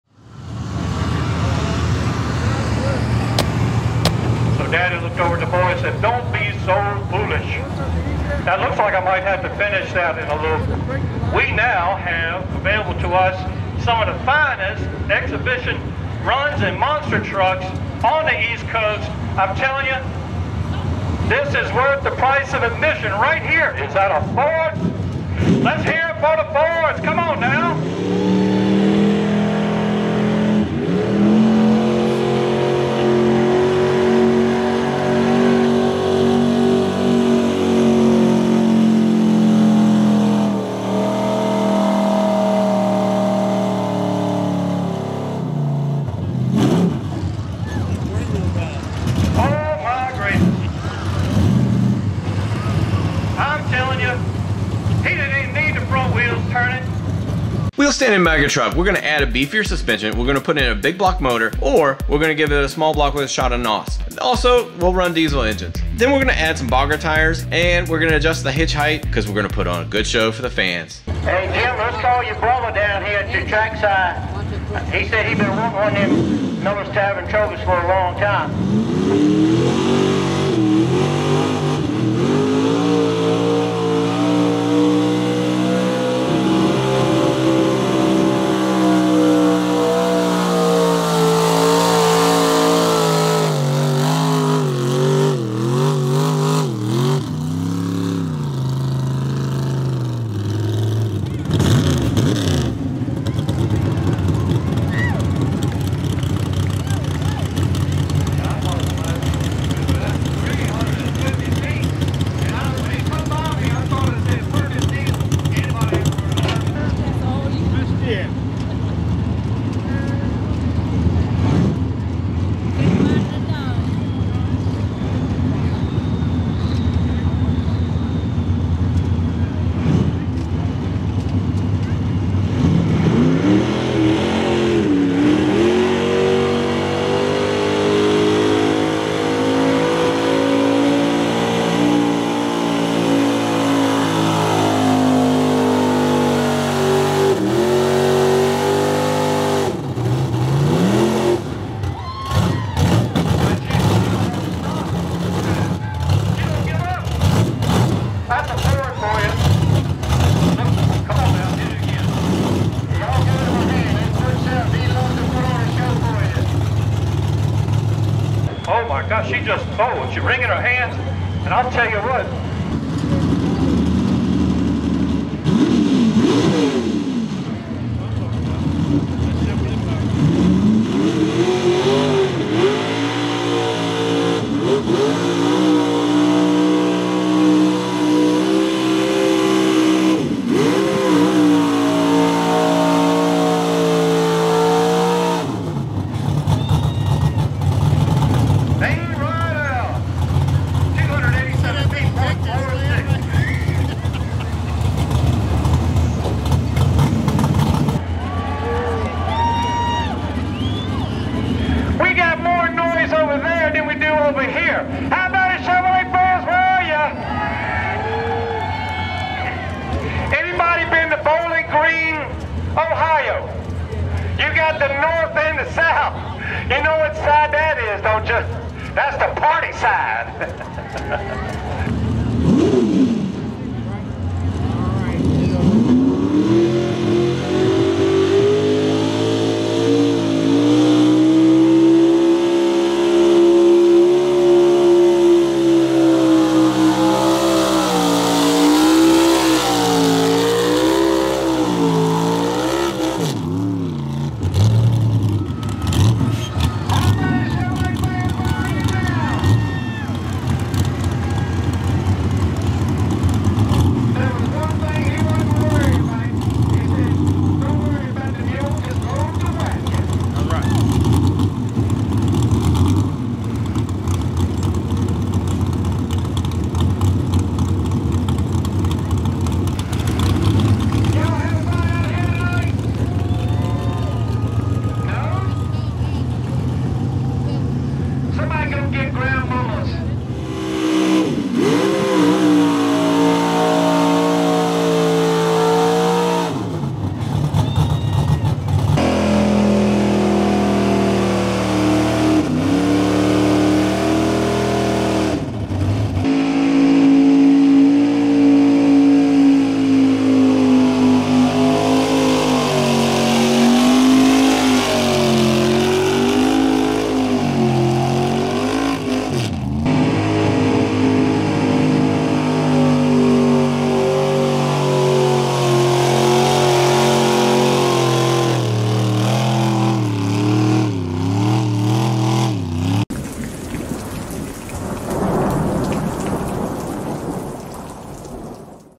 Wheel Standing Mega Trucks at Southern Showdown 2020
Davin Brann, Bain Rideout, Matt Perkins vs the Ford Boys!  Chevy fans have a good chance to put on a show but don't count these Fords out.  These pullers face off in a sled pulling competition that is about pulling, popping wheelies, and staying in bounds.  It's jacked up mega trucks from the Southern Showdown at Millers Tavern!  TRUCKS THAT BREAK THE INTERNET!!  JACKED UP CHEVYS and JACKED UP FORDS!

FeelWorld 7 Inch Field Monitor
GoPro Hero 7
Zhiyin Smooth Q Phone Stabilizer
Dell XPS 15" Laptop